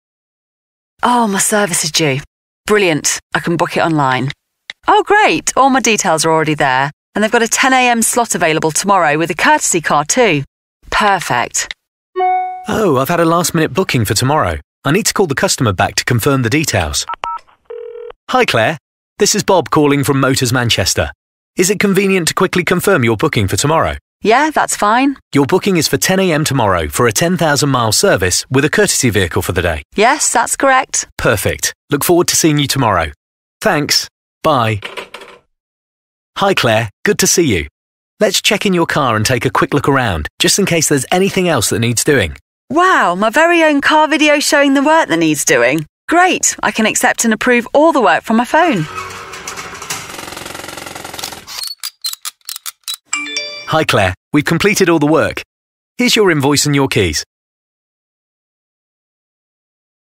CDK Global Reel!
Hey everyone! I’m thrilled to announce my collaboration with CDK Global. Trusted by nearly 15,000 dealer locations, CDK Global creates connections in automotive retail with world-class dealership software solutions. They’re all about helping dealerships reach their full potential, and I’m excited to be a part of this journey!